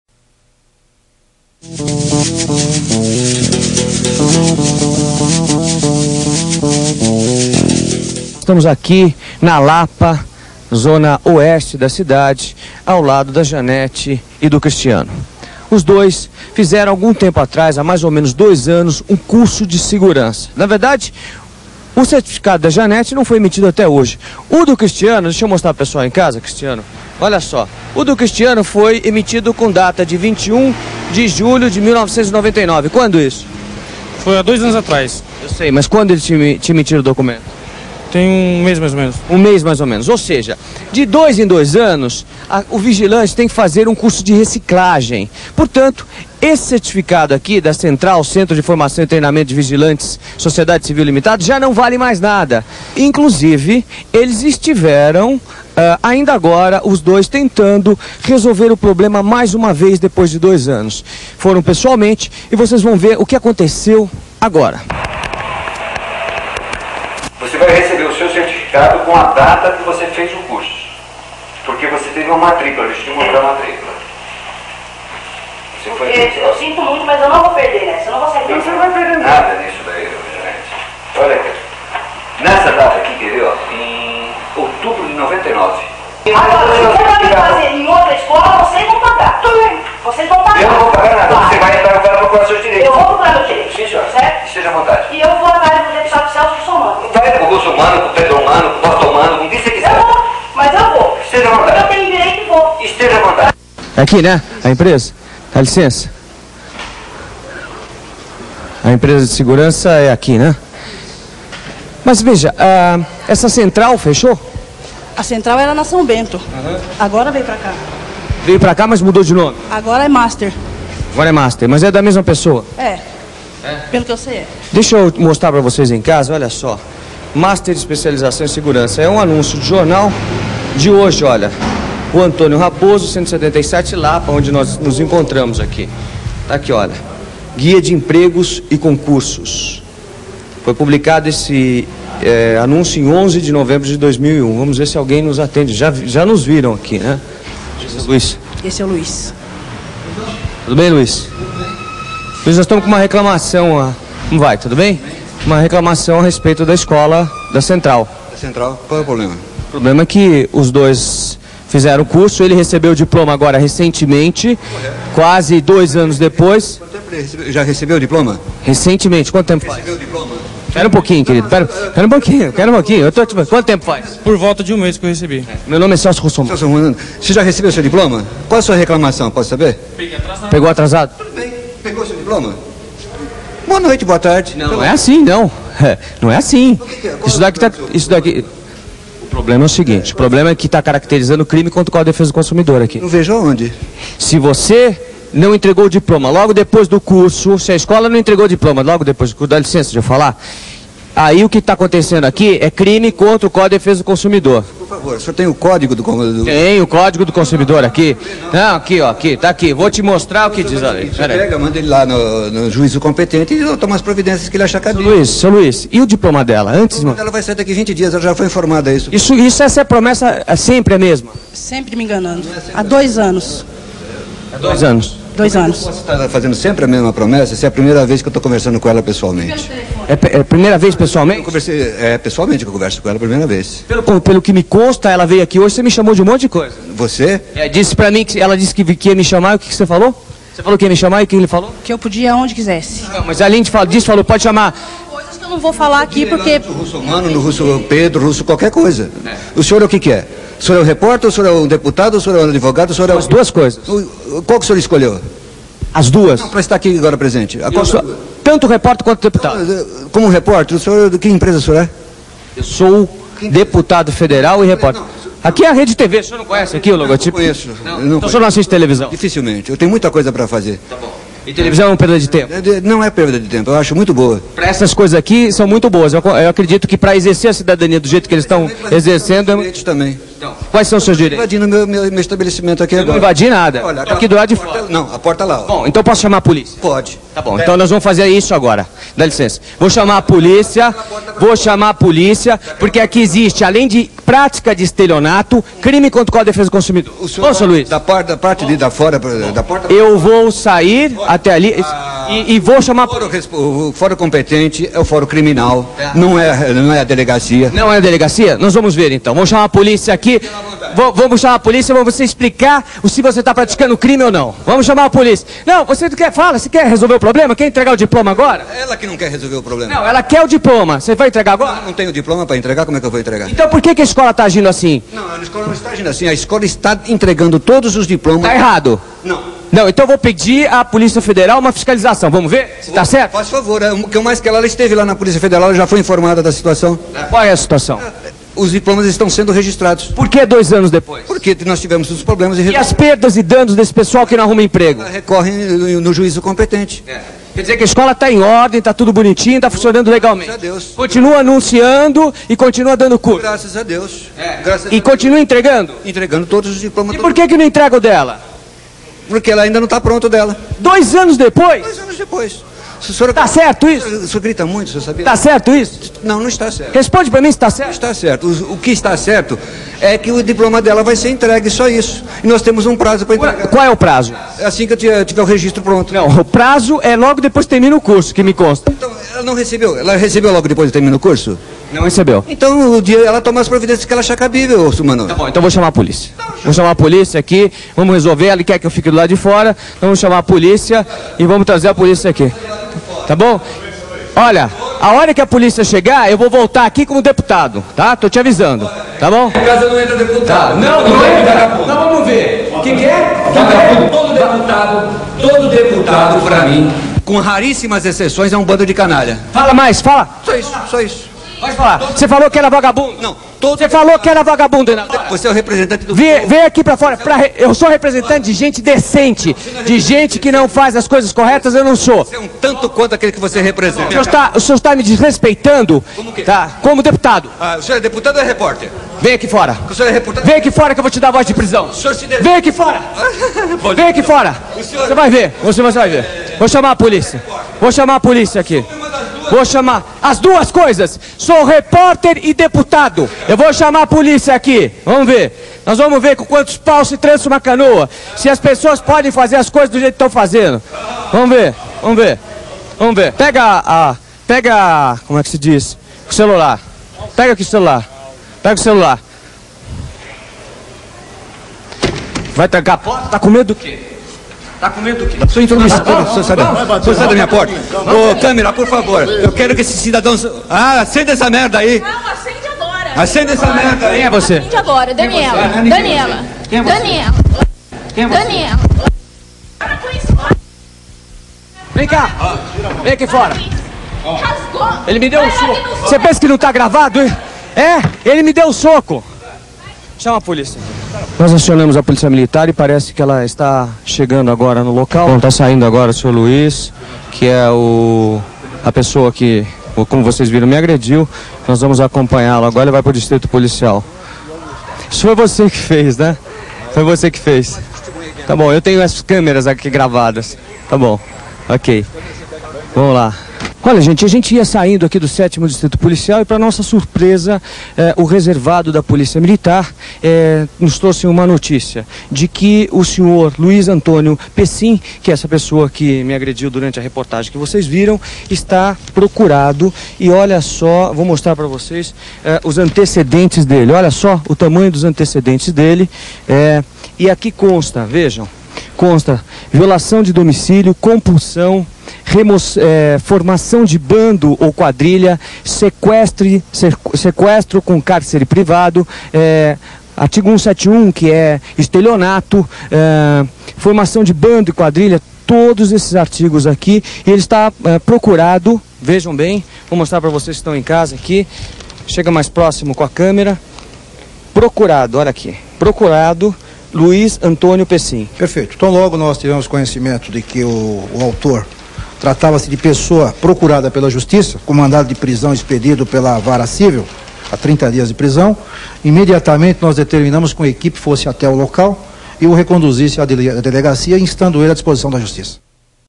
Celso Russomanno é Agredido por Defender Consumidor
Celso Russomanno foi agredido por dono de escola que achou que as câmeras estavam desligadas.